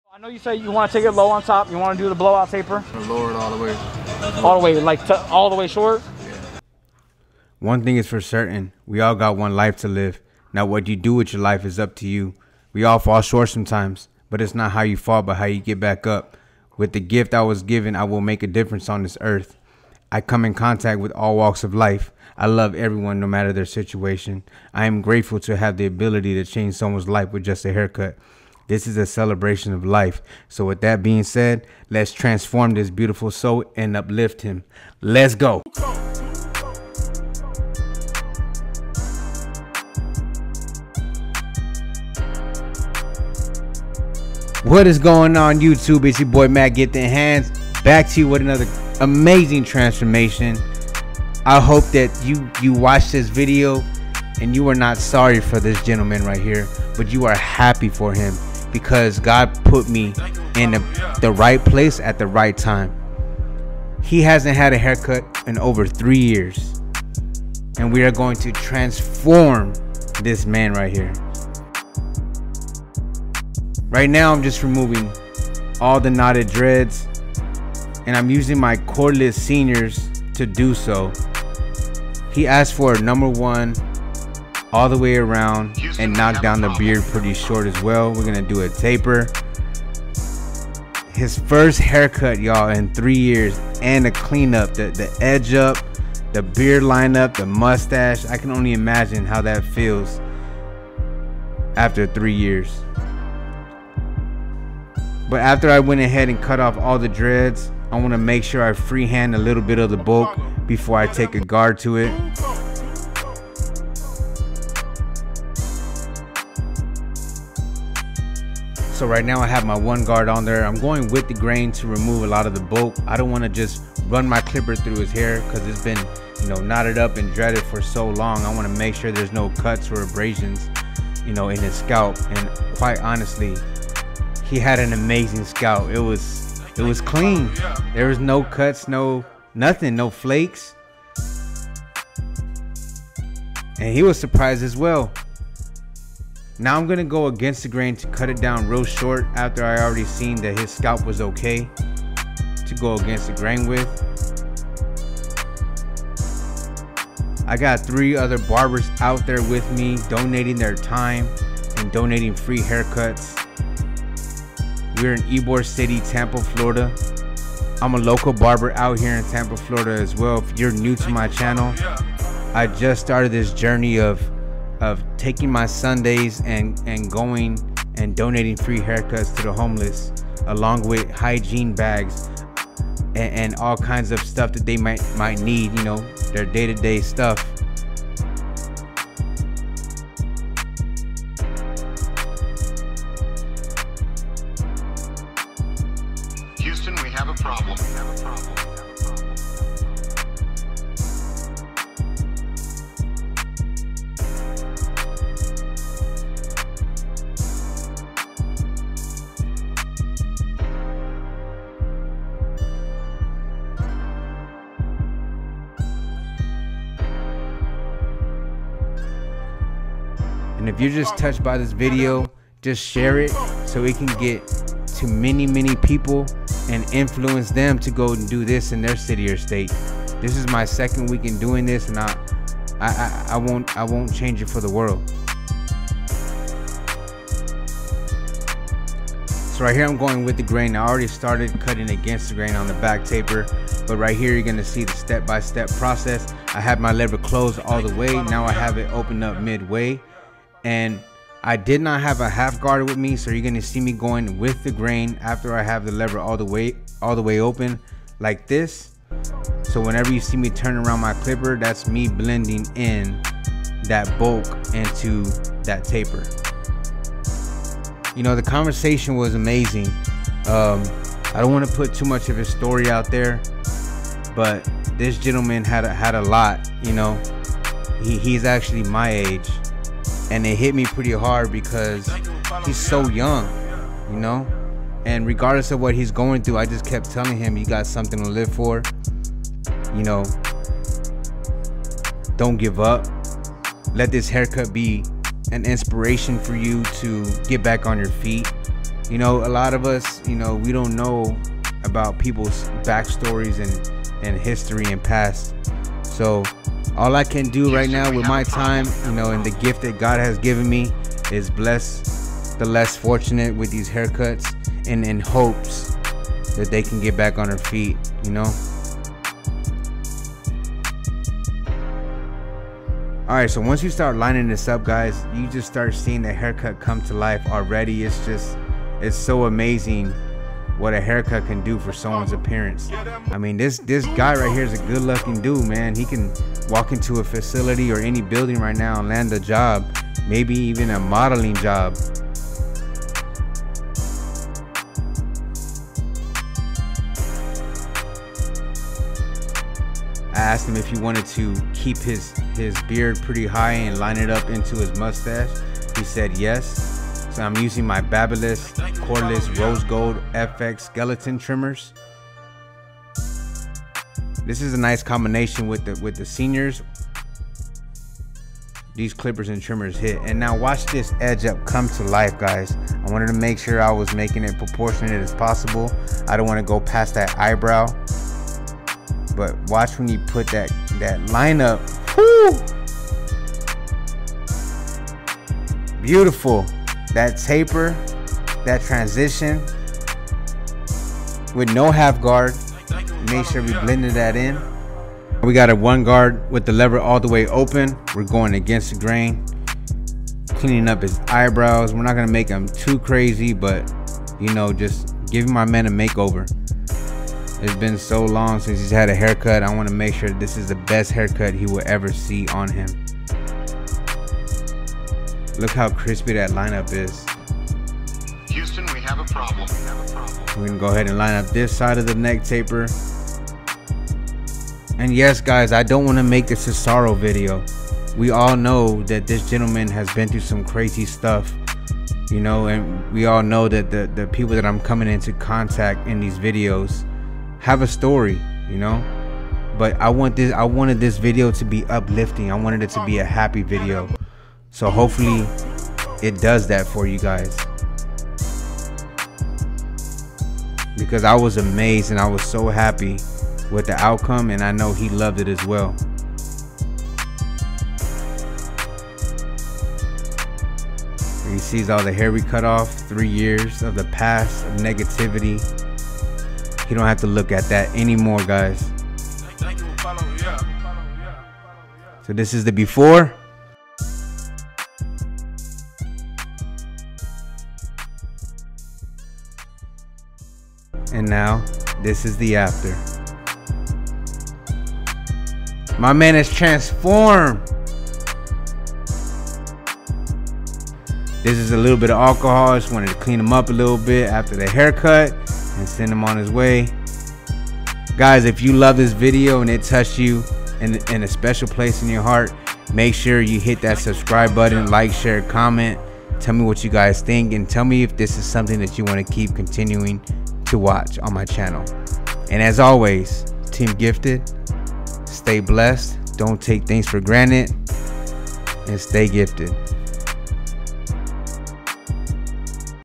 AMAZING TRANSFORMATION | 3 Years Without A Haircut | UPLIFTING
I finally did something that had been on my ❤️ for YEARS! We went downtown Tampa and hit up YBOR CITY, where We found an amazing group of guys that needed haircuts. Taking care of the homeless with what little we had was AN INDESCRIBABLE FEELING!

 I DARE YOU NOT “challenge” I DARE YOU TO GO AND DO THIS! Your life will change in MINUTES! Your opinion of your life and your struggles are nothing compared to others.

@Matt_Giftedhands On IG
OR

We can use clothes, dry foods, hygiene products, water, recyclable bottles, bags to carry their stuff in. ETC.